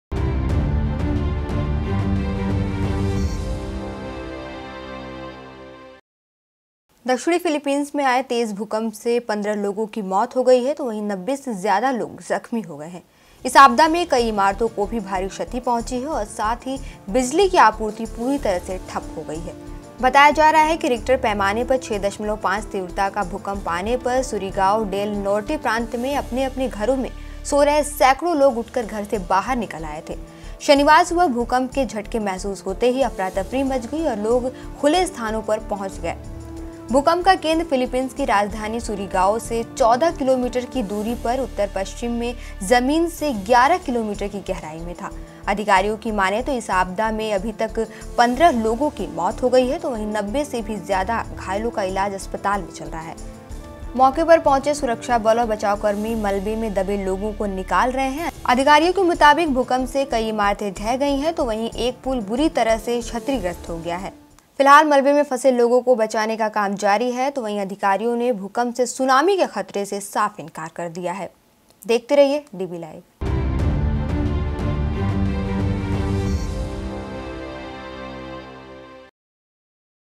DB LIVE | 11 FEB 2017 | Earthquake measuring 6.7 hits south Philippines, no tsunami warning issued
दक्षिणी फिलीपींस में आए तेज़ भूकंप से 15 लोगों की मौत हो गई है, तो वहीं 90 से भी ज्यादा लोग जख्मी हो गए हैं। इस आपदा से कई इमारतों को भारी क्षति पहुंची है और साथ ही बिजली की आपूर्ती पूरी तरह से ठप हो गई है।
बताया जा रहा है कि रिक्टर पैमाने पर 6.5 तीव्रता का भूकंप आने पर सुरिगाओ डेल नोर्टे प्रांत में अपने-अपने घरों में सो रहे सैंकड़ो लोग उठकर घर से बाहर निकल आए थे। शनिवार सुबह भूकंप के  झटके महसूस होते ही अफरातफरी मच गई और लोग खुले स्थानों पर पहुंच गए। भूकंप का केद्र फिलीपींस की राजधानी सुरिगाओ से 14 किलोमीटर की  दूरी पर उत्तर-पश्चिम में जमीन से 11 किलोमीटर की गहराई में था। अधिकारियों की मानें तो इस आपदा में अभितक 15 लोगों की मौत हो गई है तो वहीं 90 से भी ज्यादा घायलों का इलाज अस्पताल में चल रहा है। मौके पर पहुंची सुरक्षाहल और बचावकर्मी मलबे में दबे लोगों को निकाल रहे हैं।अधिकारियों के मुताबिक भूकंर से कई इमारते ढह गई हैं तो वहीं एक पुल बुरी तरह से क्षतिग्रस्त हो गया है।
फिलहाल मलबे में फंसे लोगों को बचाने का काम जारी है। तो वहीं अधिकारियों ने भूकंप से सुनामी के खतरे से साफ इंकार कर दिया है।..देखते रहिए डीबी लाइव।